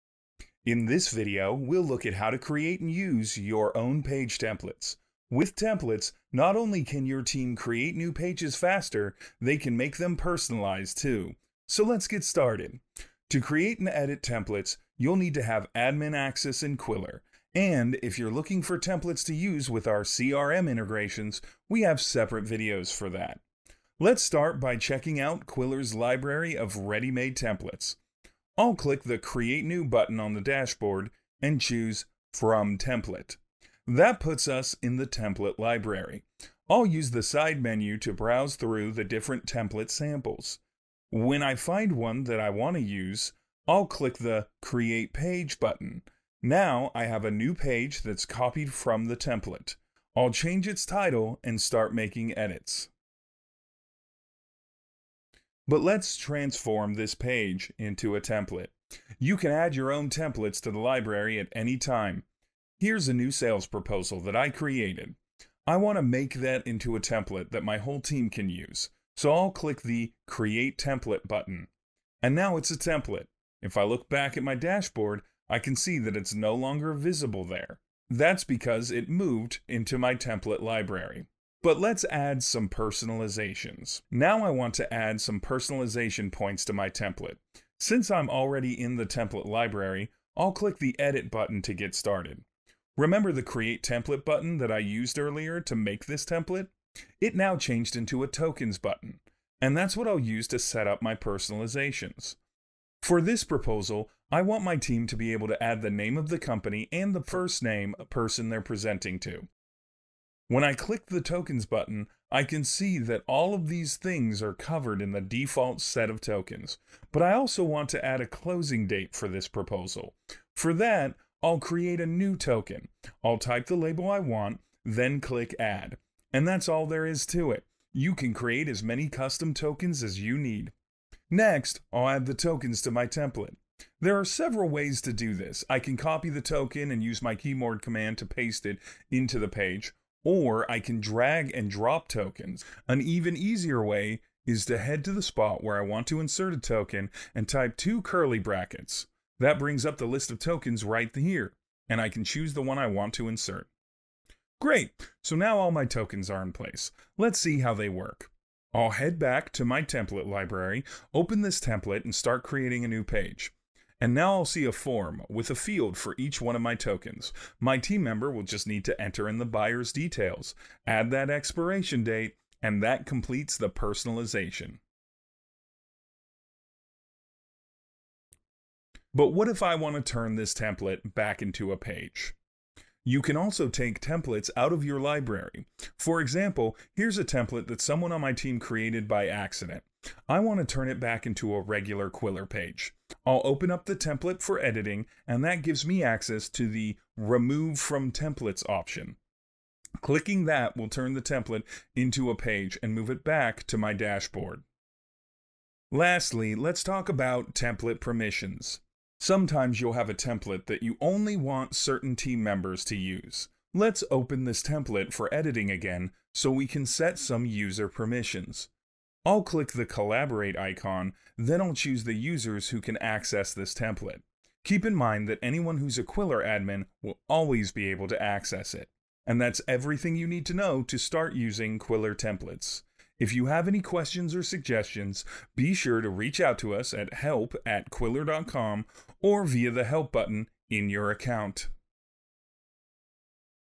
Qwilr Tutorial: Creating & Managing Templates
Qwilr helps you create sales experiences that wow buyers and drive revenue.

In this tutorial video, we’ll learn how to you can build templates that help you create personalized pages with just a few clicks.

Check out our blog for better sales experiences, insights, and value